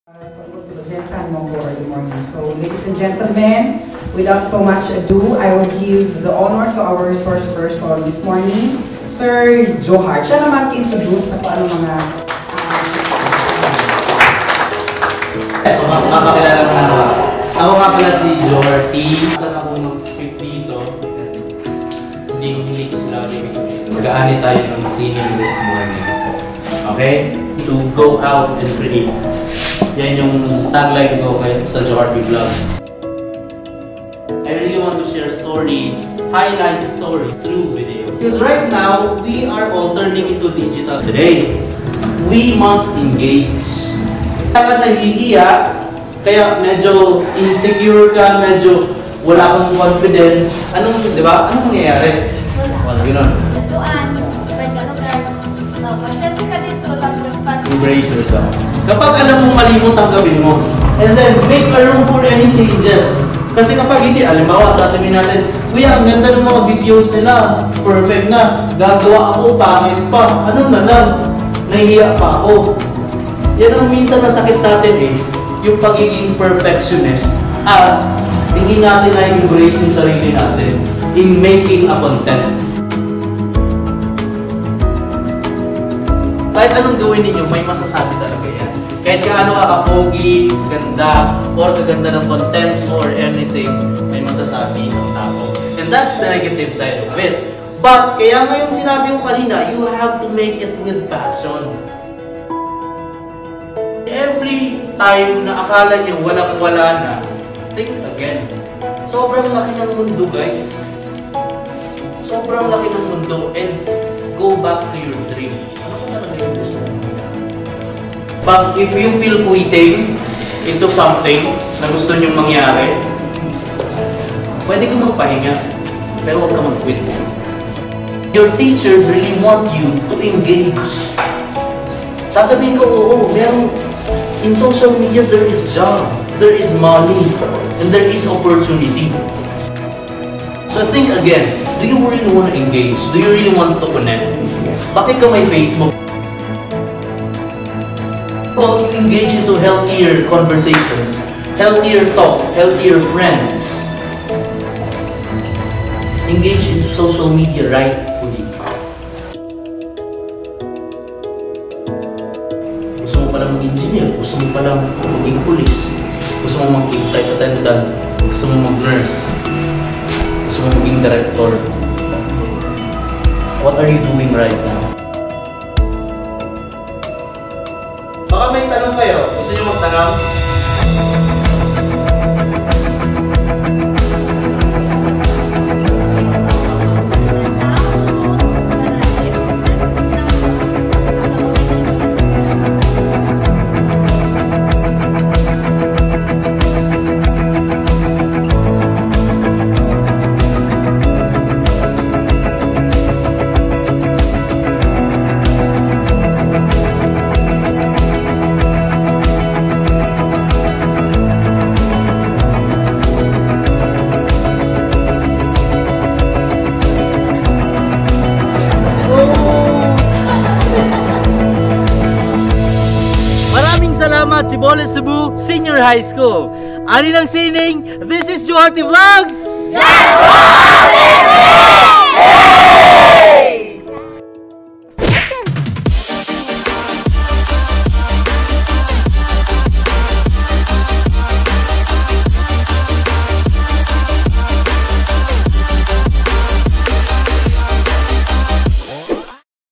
National Arts Month 2020 - Vlogging Workshop
National Arts Month 2020
A Vlogging Workshop with Joharty Vlogs as CameraMan ng GenSan

Thank you LGU Lake Sebu through the leadership of Mayor Floro Gandam Sr.. and the ever energetic President of Lake Sebu Culture and Arts Council Ms. Nenita Ganlal Kinan

This undertaking is made possible through collaborative effort of LGU Lake Sebu, NCCA T'boli/Ubo Cluster and Tboli Sbu Senior High School.

and ofcourse Mr. Michael Angelo Yambok

Thank you for the opportunity and the warmth of Love that you all showed, specially for the students of Sbu National HighSchool. See you sa gala natin soon...